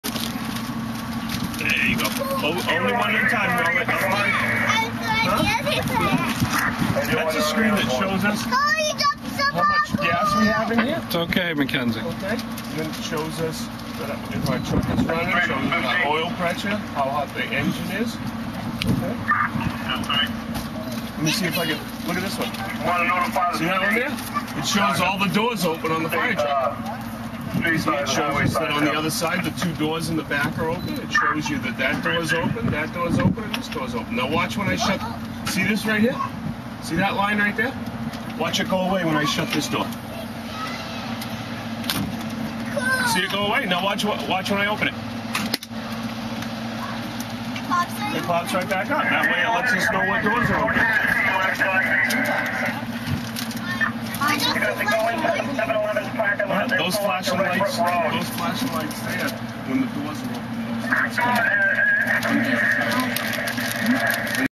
Declan and Cole in a Fire Truck
The Fireman explains the Truck to Declan and Cole